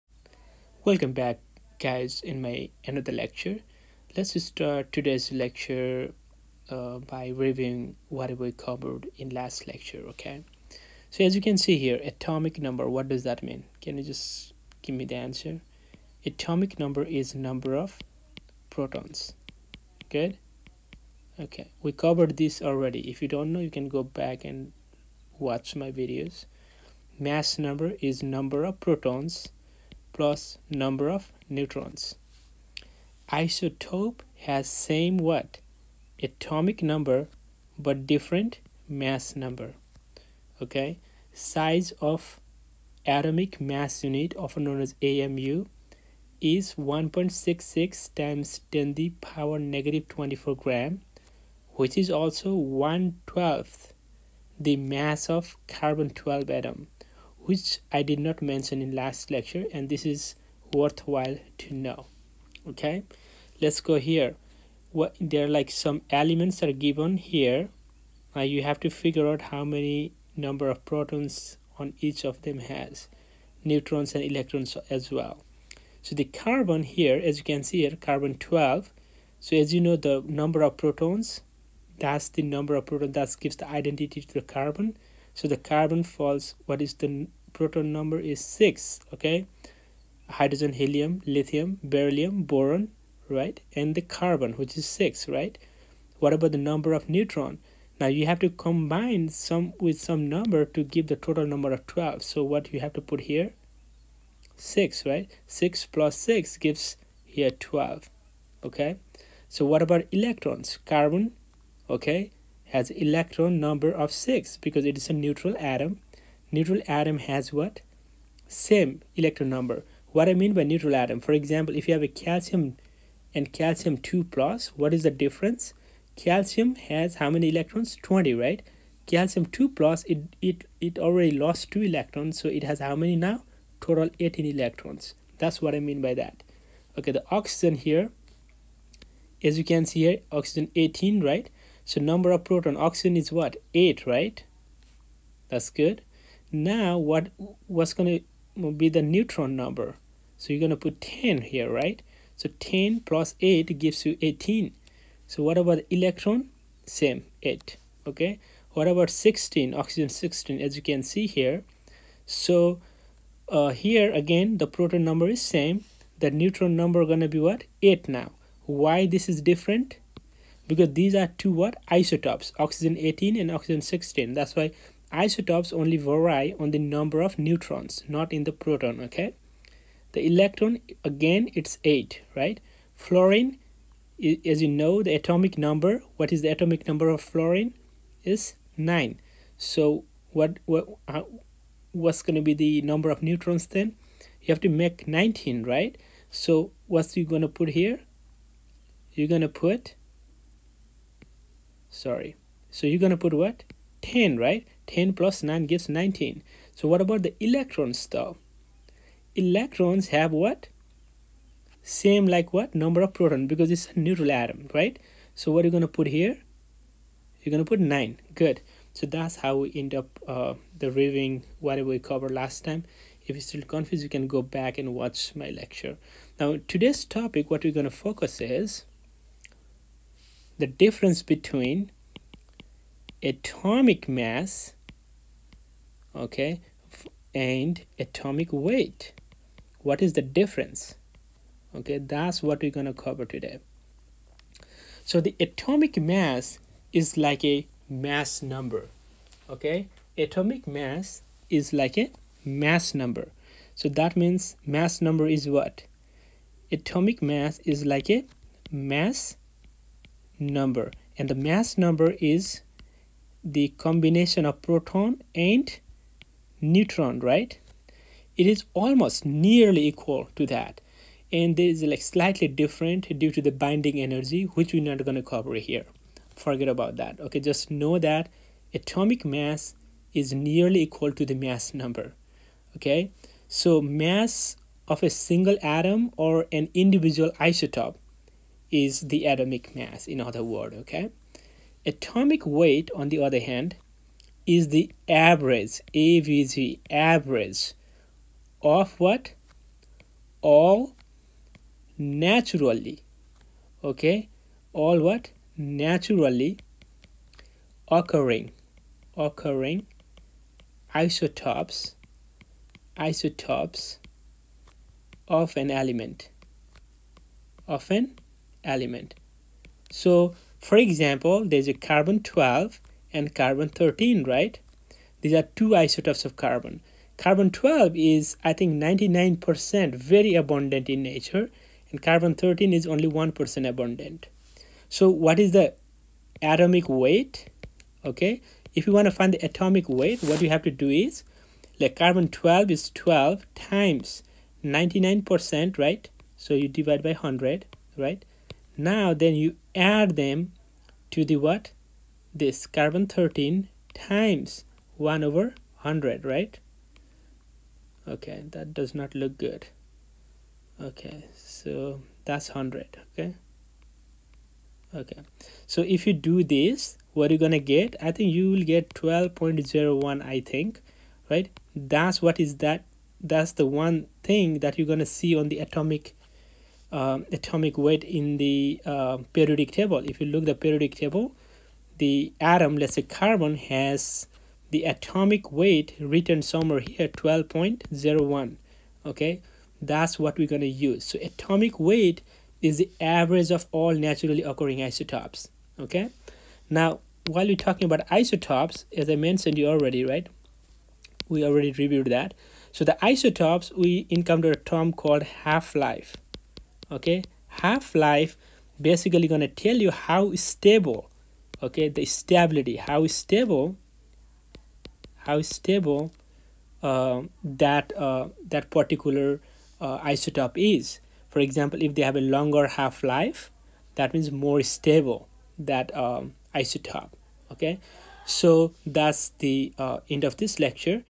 Difference Between Atomic Mass And Atomic Weight
Atomic mass is nearly equal to mass number, which is (protons + neutrons). The atomic weight is the average weight of all naturally occurring isotopes of that particular element. For example C-12 and C-13 are two isotopes of carbon. The average of both isotope.gives an atomic weight of 12.01 g. In this video, I forgot to divide two isotopes with 2 to get average. I hope this clarifies any misconceptions.